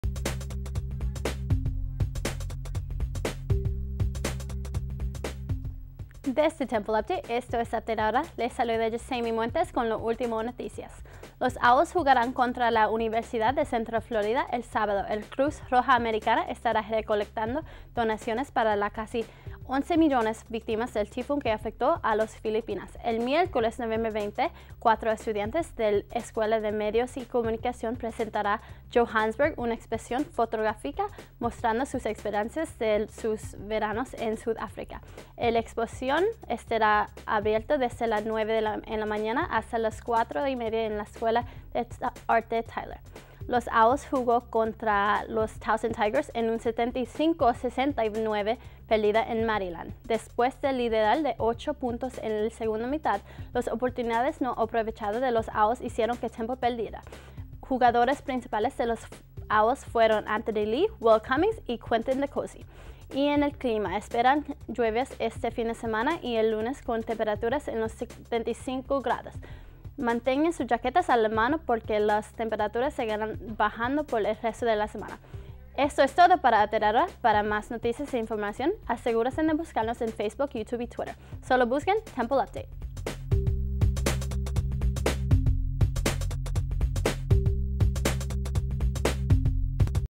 Update Ahora; Noviembre 15, 2013
Desde el cuarto de noticias de Temple Update, esto es Temple ahora. Aqui estan las ultimas noticias.
- Recaudacion Para Victimas de Filipinas
- Exposicion Fotografica "Johannesburg"
- Baloncesto de Hombres Contra Towson